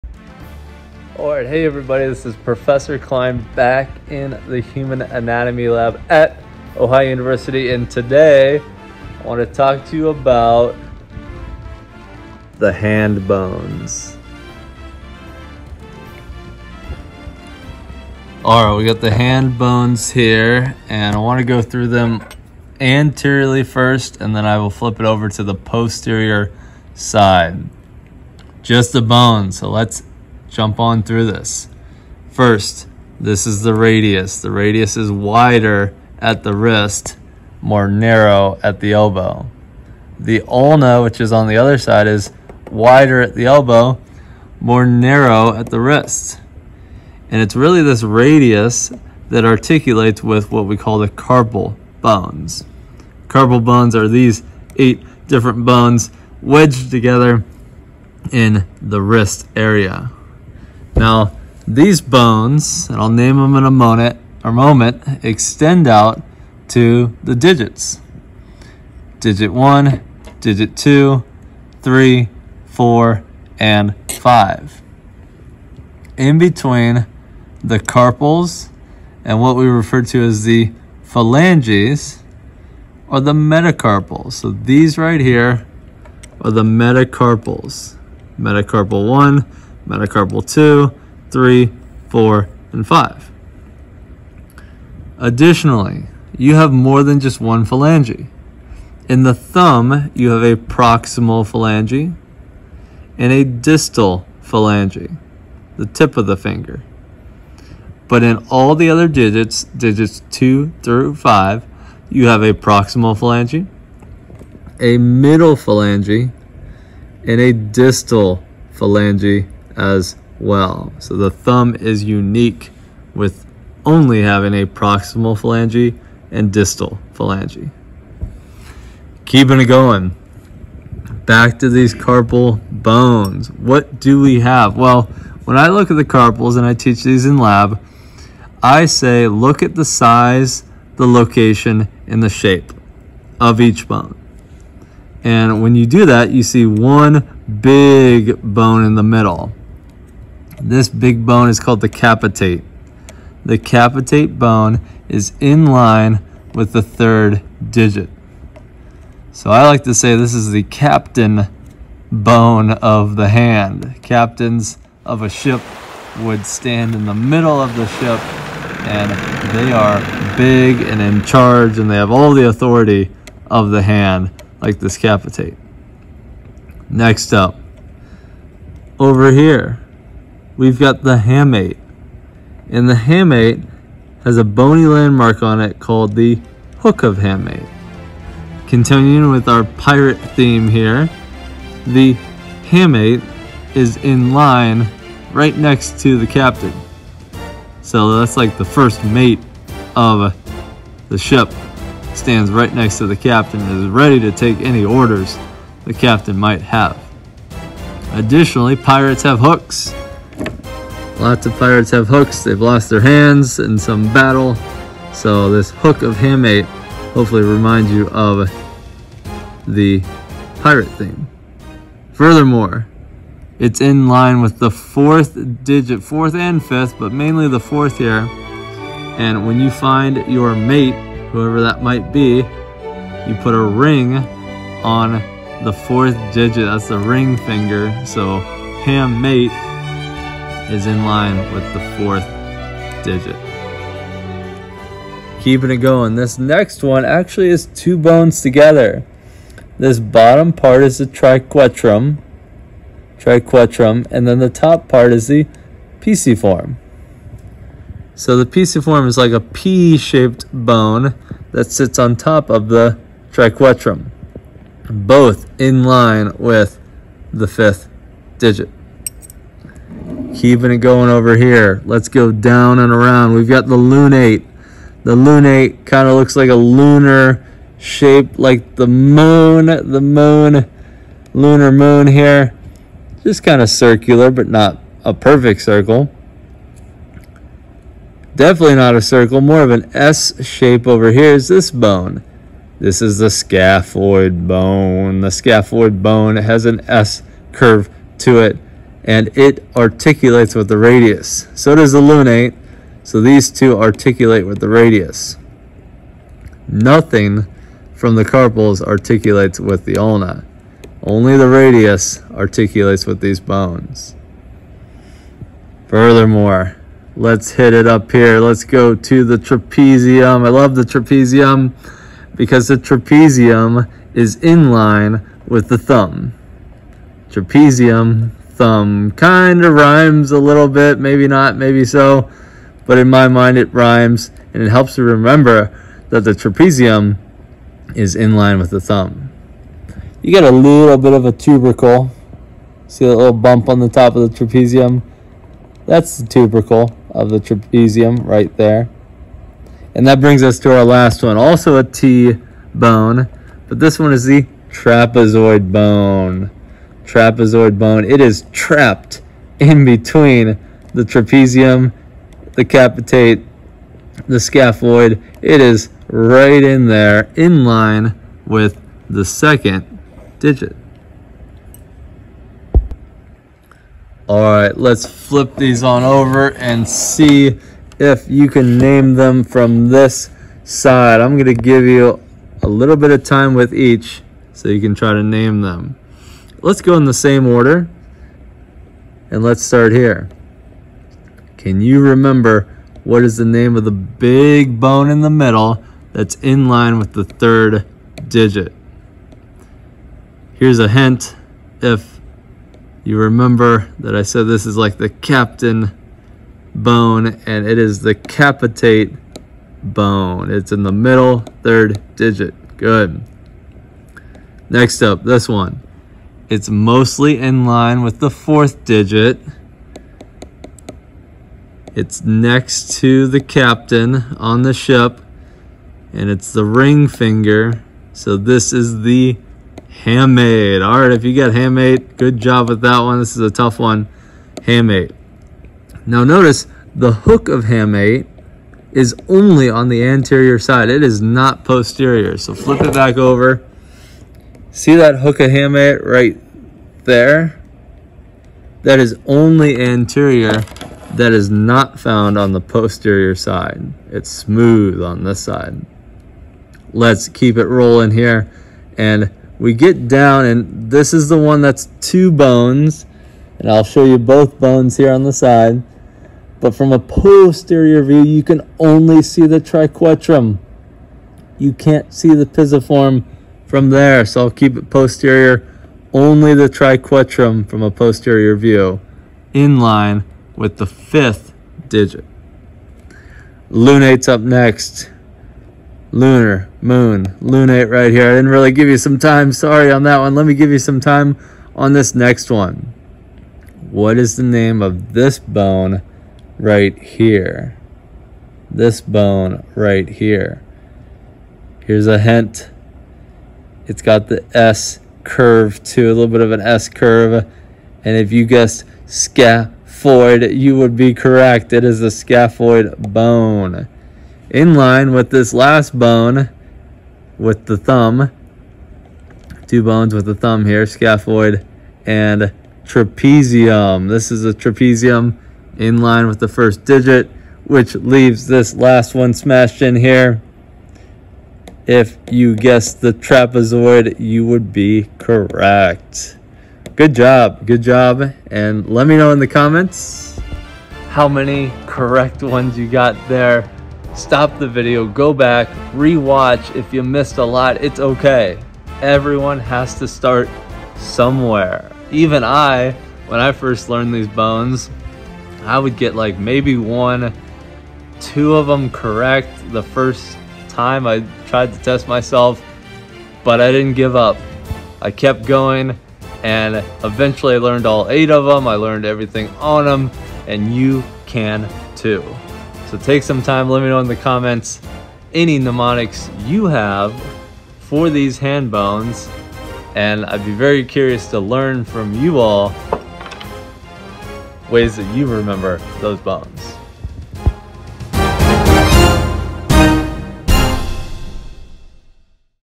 Hand Bones (Best Analogy Ever!)
Hand Bone Video that details:
Wrist
Hand
Carpal Bones
Analogies for remembering them all!